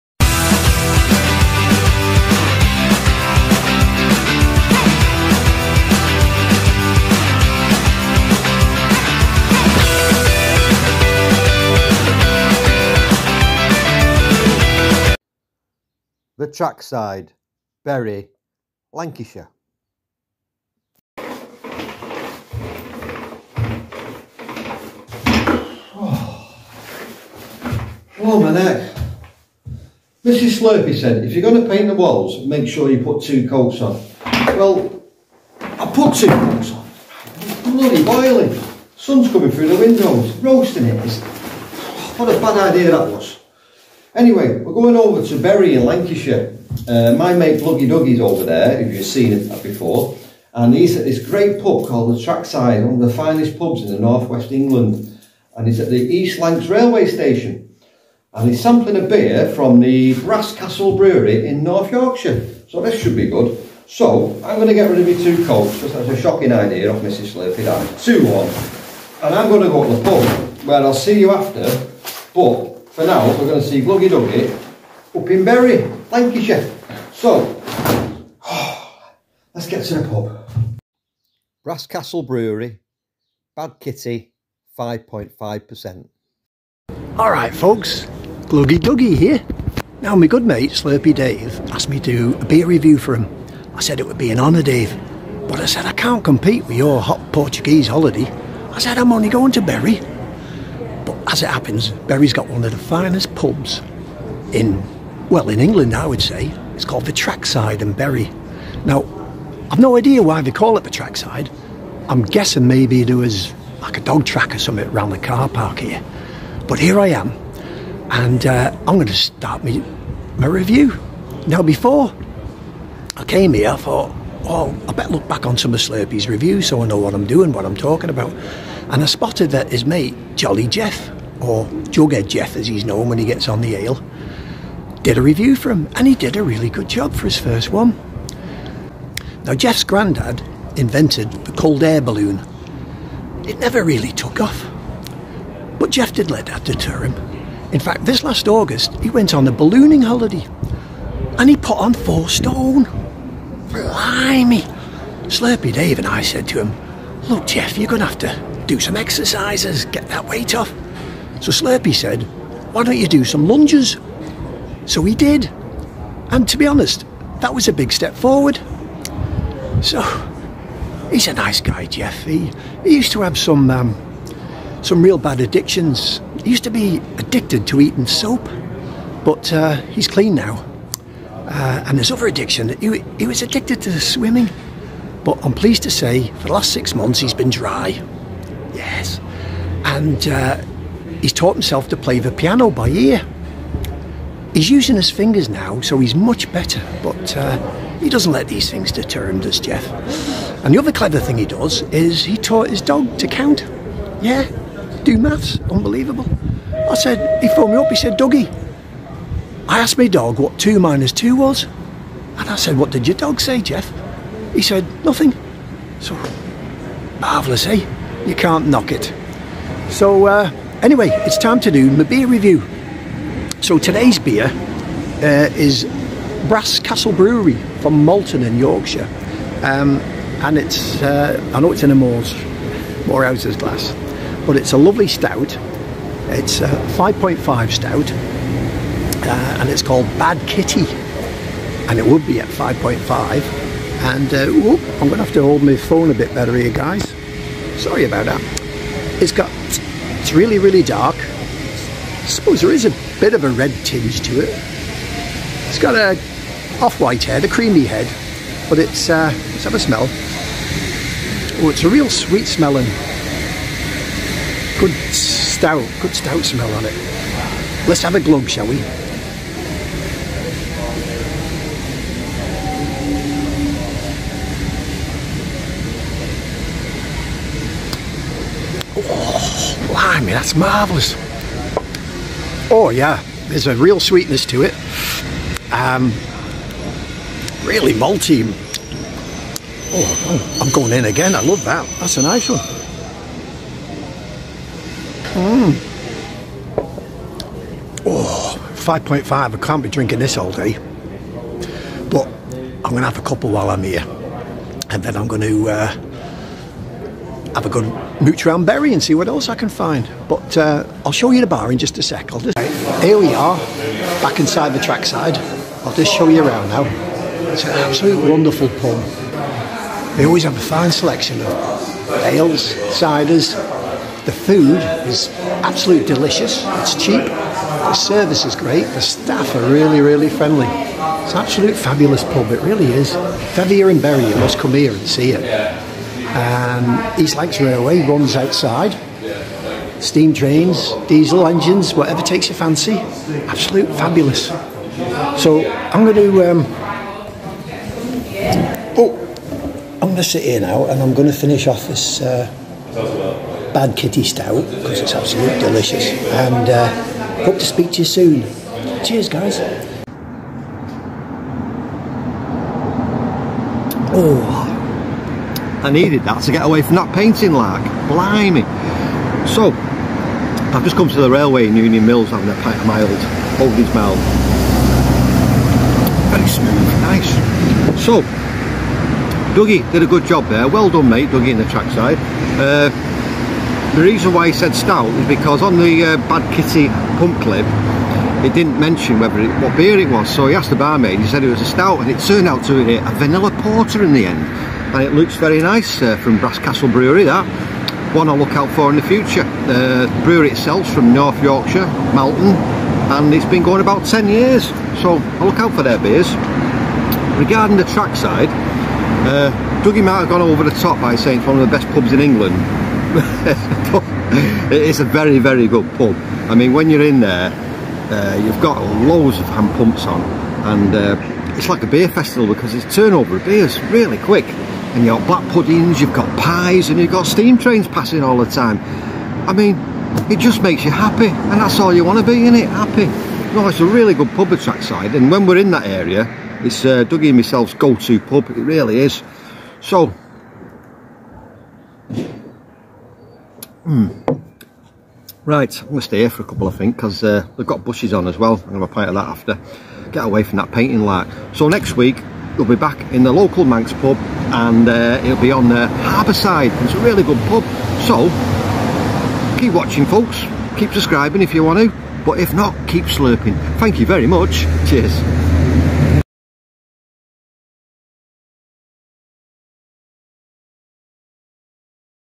Weekly (NOT Manx) , Pub & Cask Ale Review. Trackside, Bury, Lancs. (Brass Castle Bad Kitty Porter)
Weekly (NOT Manx), Pub & Cask Ale Review

PUB...The Trackside,  Bury, Lancashire

BEER ...Brass Castle Bad Kitty Porter 5.5%

BREWERY...Brass Castle, malton, N.Yorkshire, England

ESTABLISHED 2011